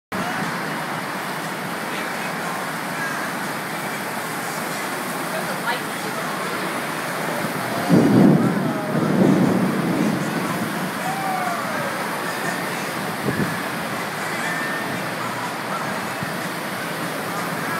Slugger Field Rain Delay
We had an awful thunderstorm here in Louisville, KY August 13th & I was at Slugger Field when it came through.